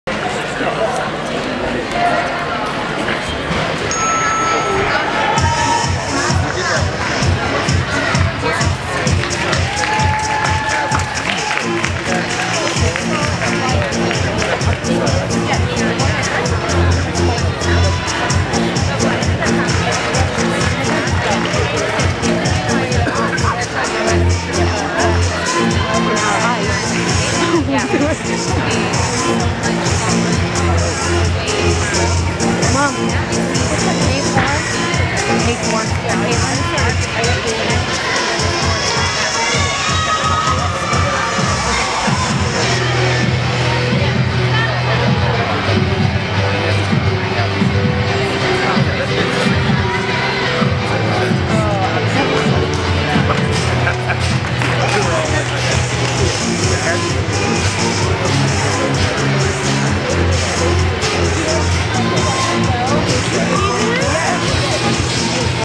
2018 Town & Country Hotel San Diego Meet - Vault - Xcel Silver - Natalie Aguirre
Gliders 2018 - Charter Oak Gymnastics
Xcel Silver - 9 years old

2018 San Diego Meet Town & Country Hotel
Sunday, January 28, 2018
12:00 PM
Town & Country Hotel Convention Center